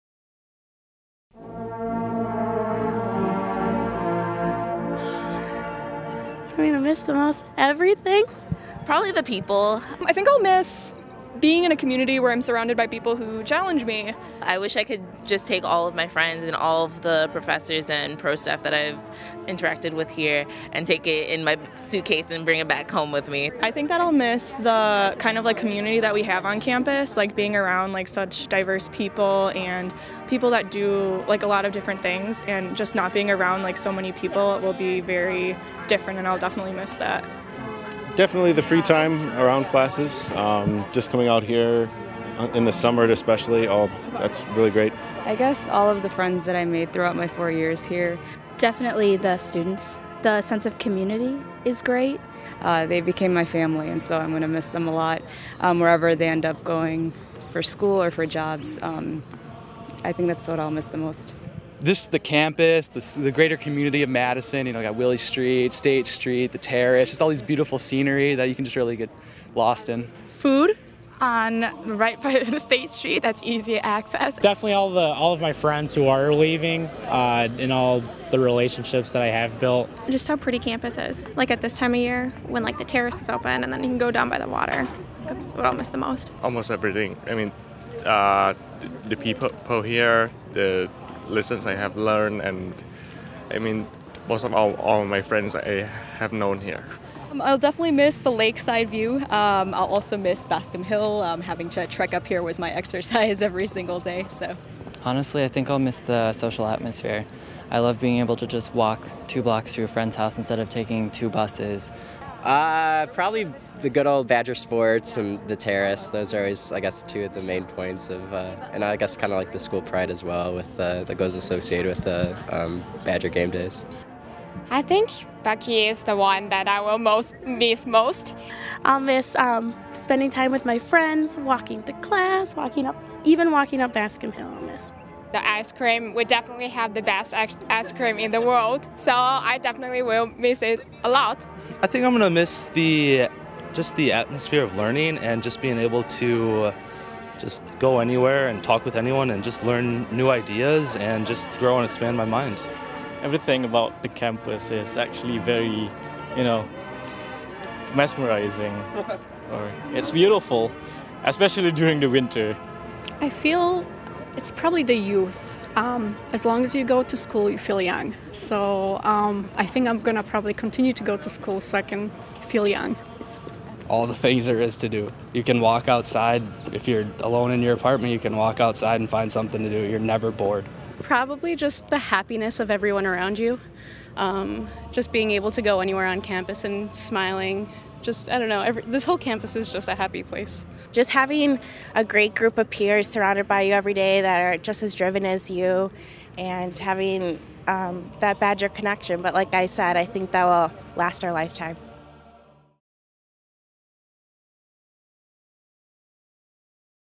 What will you miss the most when you leave UW-Madison?
We asked graduating seniors what they will miss the most about their time here at UW-Madison.